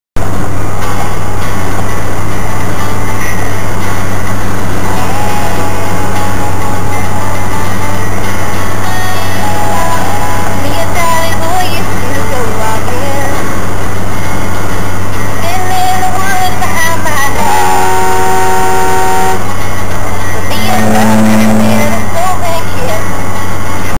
near miss at 75 mph
passing a car that for some reason didnt see me. i had my cruise on 75mph so all the speeding up and slowing down is on her part